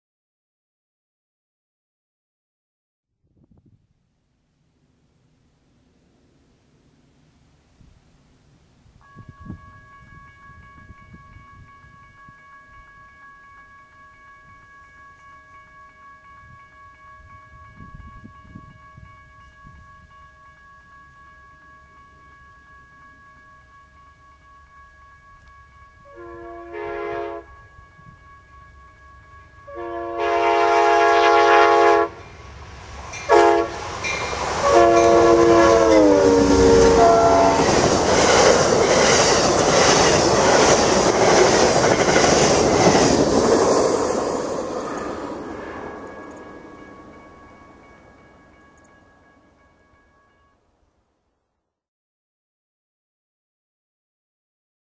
Amtrak's Southwest Chief #3 in Galisteo, NM
Here is a shot of Amtrak's westbound Southwest Chief from back in December of 2011, despite the lack of snow; it was an extremely cold afternoon. The train has just minutes earlier, made an on-time departure from Lamy, NM and is now traveling at nearly full speed as it continues west towards Albuquerque, NM. There are two locomotives powering today's westbound Chief, both are P42s: #79 leading with #64 trailing. Recorded on December 10, 2011.

SWChief34